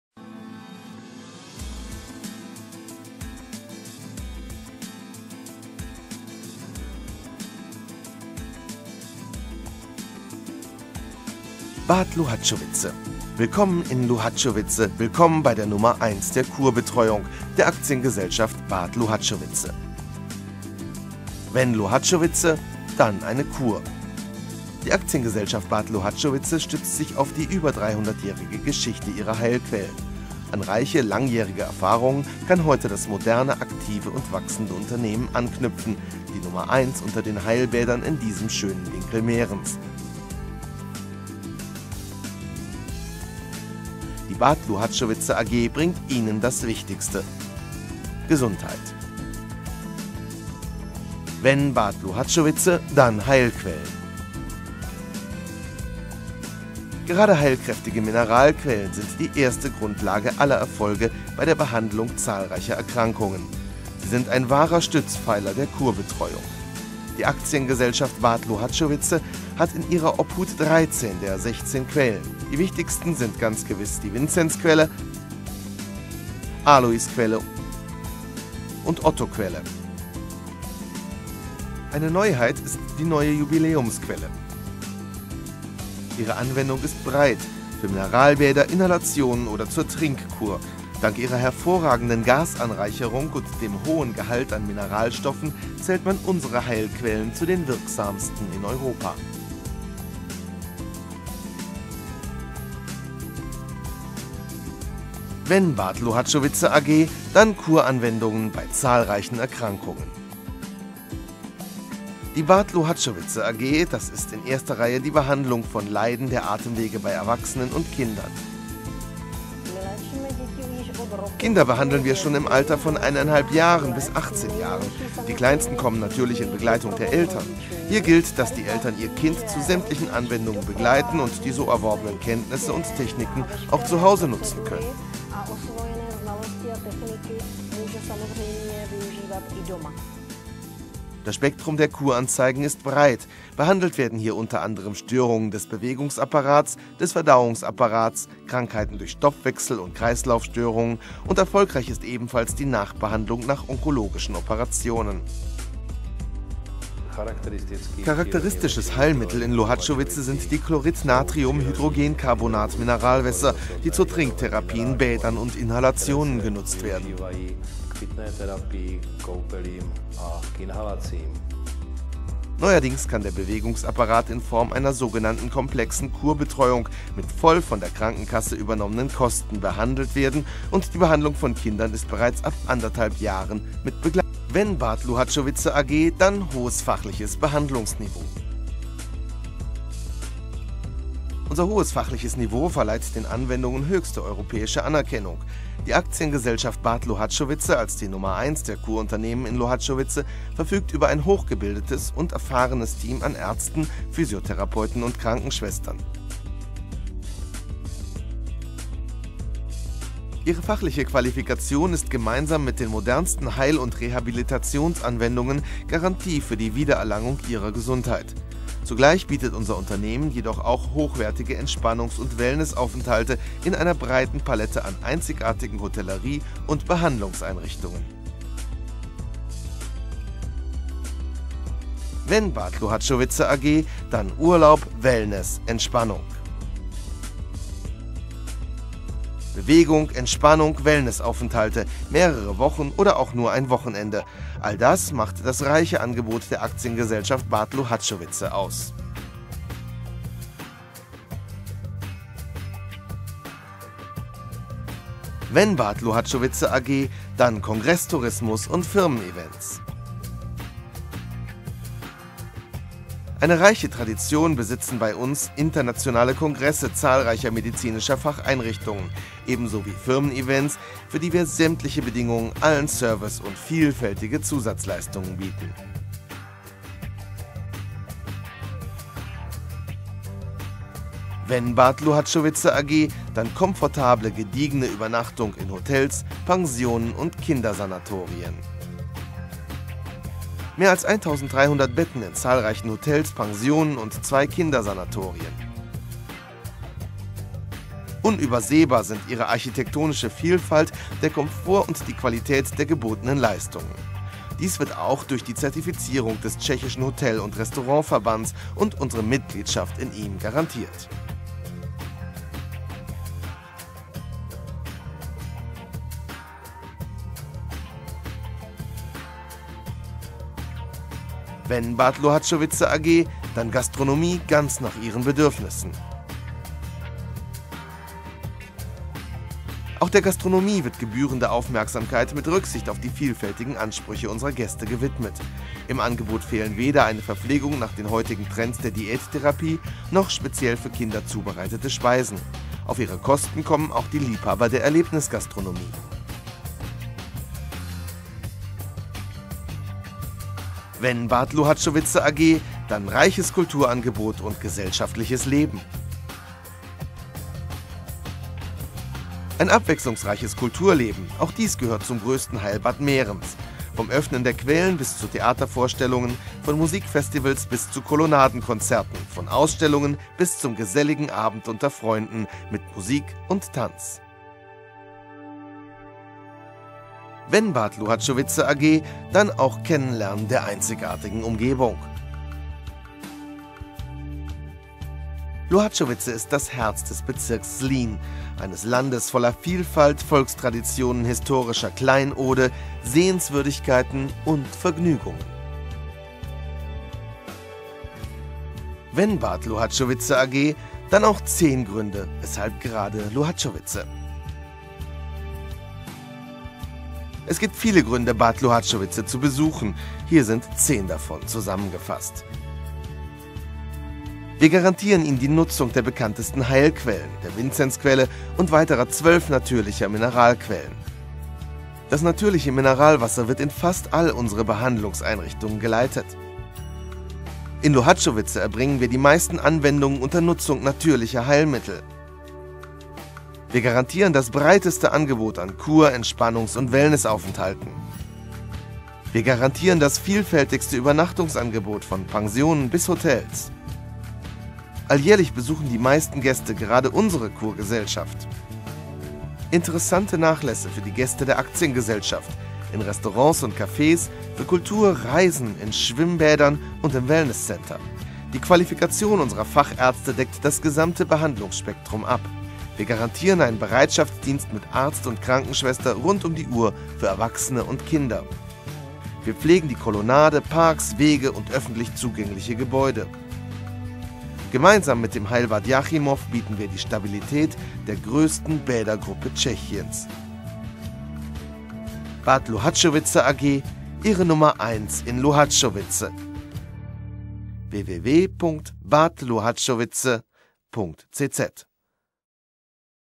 Heilbad Luhačovice, Tschechische Republik
Das Bad Luhačovice verdankt seinen Ruhm nicht nur der 300-jährigen Tradition der Kurbehandlungen, dem Zauber der Architektur unserer Kurhäuser, den gepflegten Parks und der Schönheit der umliegenden Natur, sondern vor allem seinen Heilquellen.